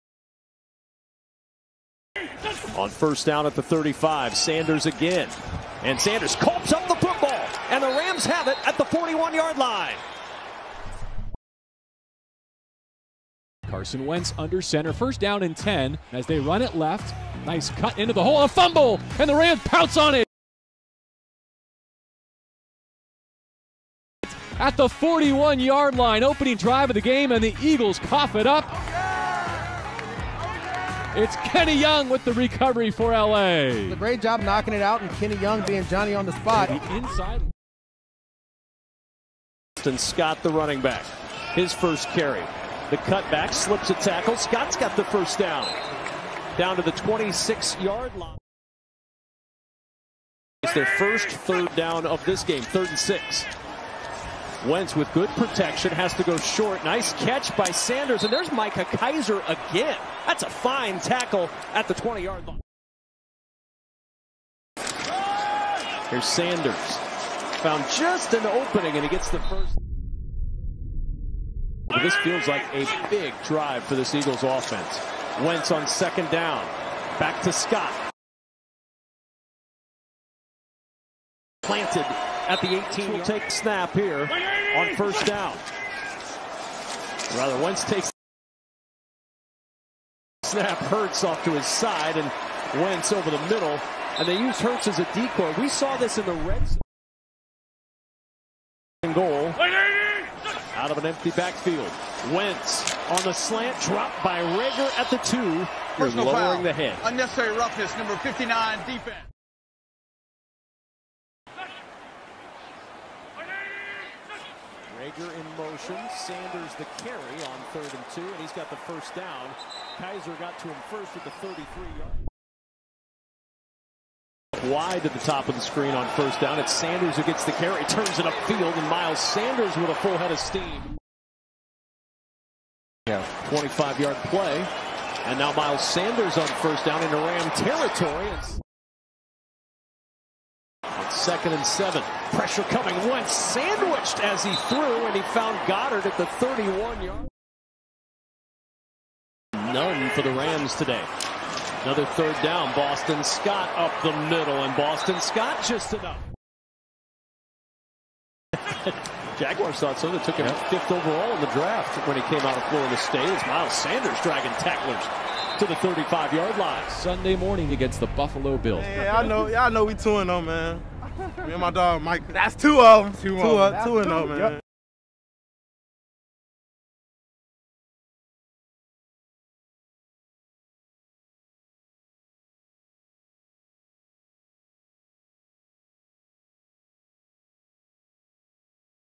Micah Kiser Highlights Week 2 | Rams vs Eagles NFL Highlights Week 2 | 16 Tackles | 2020
Rams did a great job on defense vs the Philedelphia eagles and Micah Kiser contributed plenty torwards it racking up 16 total tackles combined and forcing a turnover w/ the forced fumble recovered by the Los Angeles Rams.

Rams vs Eagle NFL Highlights Week 2.
NFL Season 2020